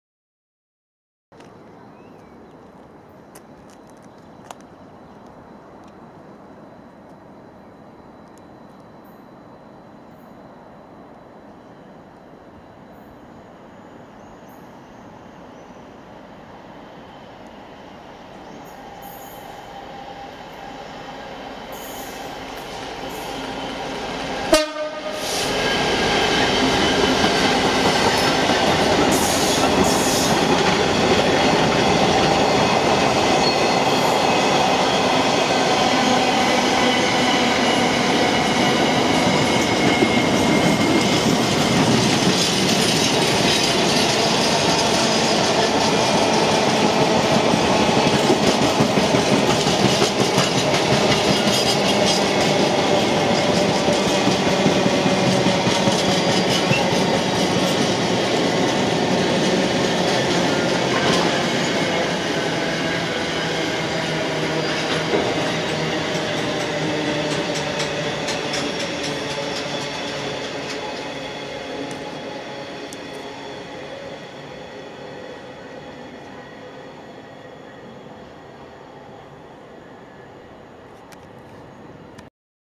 DBS 66131 on 4M83 London Gateway Dbschenker to Trafford Park Euro Term Passing Barking 08/06/2014
DBS 66131 on 4M83 London Gateway Dbschenker to Trafford Park Euro Term Passing Barking 08/06/2014 with tones